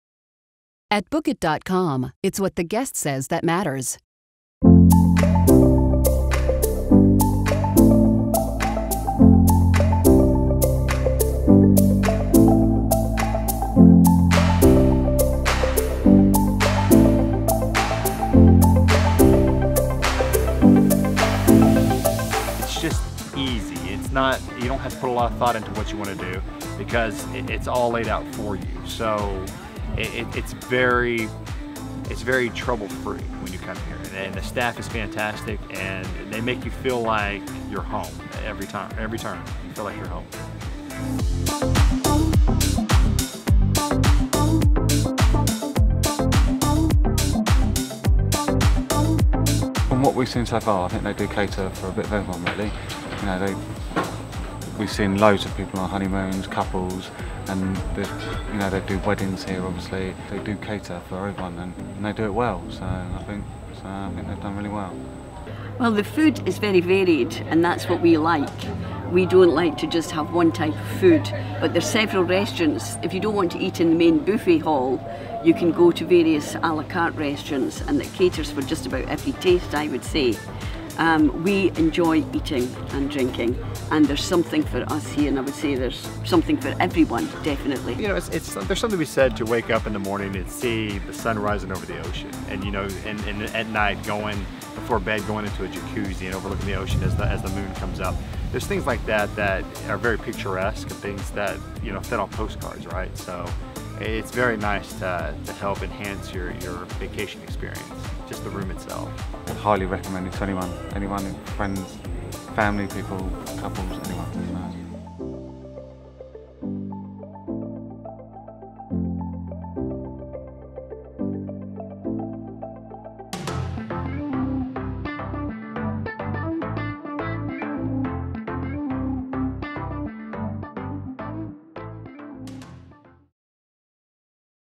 Now Sapphire Riviera Cancun | BookIt.com Guest Reviews
Now Sapphire is an upscale, all-inclusive beachfront resort that is located half an hour south of Cancun, near the quiet fishing village of Puerto Morelos. The entire family will love this resort that stretches along a lush tropical mangrove preserve on a 5,000 foot Caribbean coastline. The pristine beach is protected by the second largest coral reef formation in the world. The resort features Mayan architecture and an Unlimited-Luxury plan, providing you with all the amenities to make your perfect vacation complete.